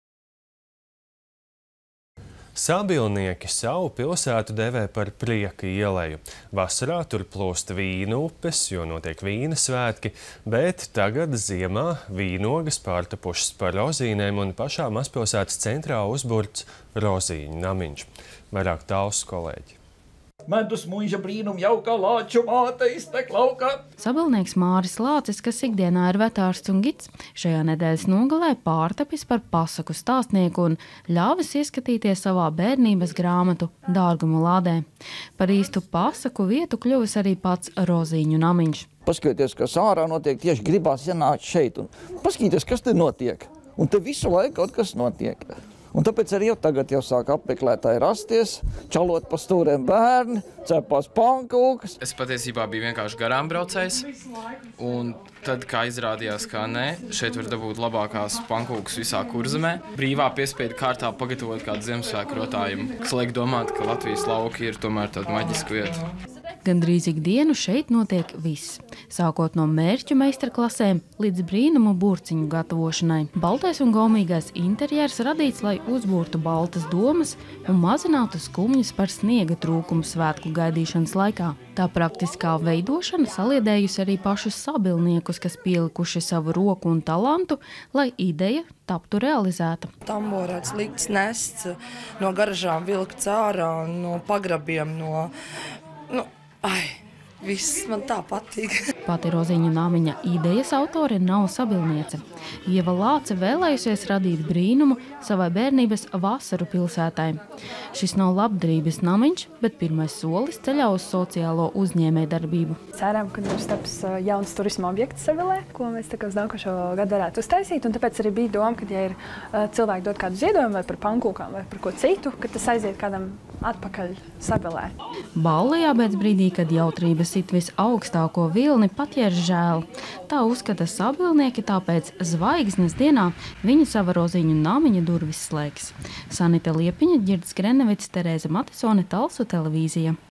Sabilē tapis Rozīņu namiņš
Sabilnieki savu pilsētu dēvē par Prieka ieleju. Vasarā tur plūst vīna upes, jo notiek Vīna svētki, bet tagad, ziemā vīnogas pārtapušas par rozīnēm un pašā mazpilsētas centrā uzburts Rozīņu namiņš.  Vairāk TV Kurzeme.